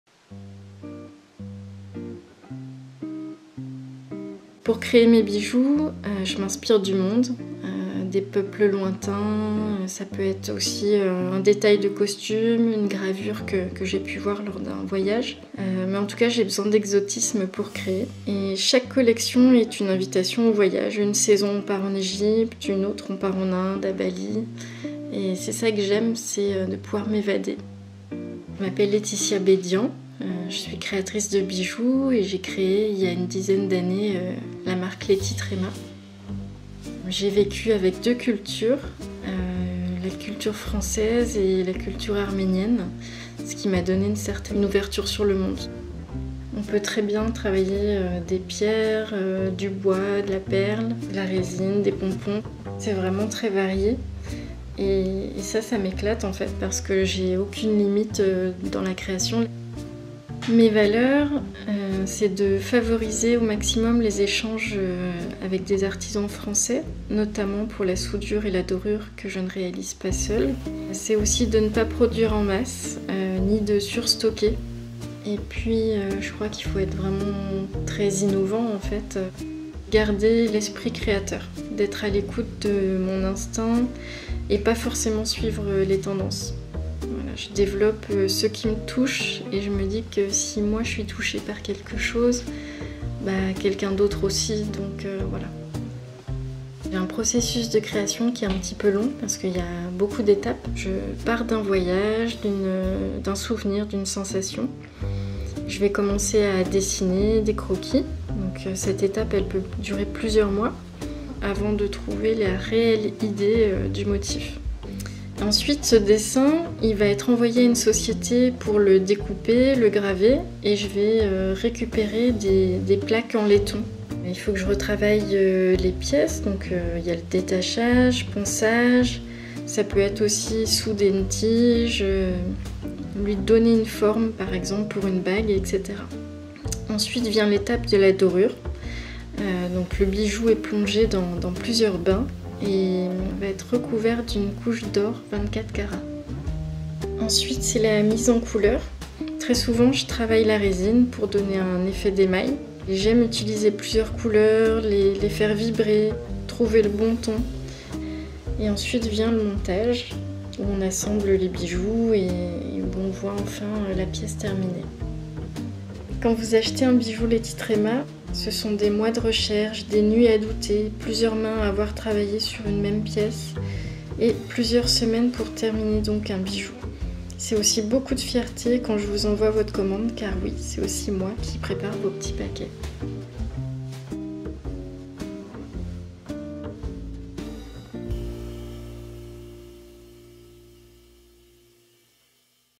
DANS L'ATELIER DE LAËTI TRËMA, CRÉATRICE DE BIJOUX
Hello la communauté !
Aujourd'hui, je partage une vidéo que j'ai réalisée pour LAETI TREMA Bijoux, créatrice de bijoux, à Brandivy (Morbihan).

Vous voulez réaliser une vidéo pour booster votre activité ? Prenons un peu de temps pour en discuter !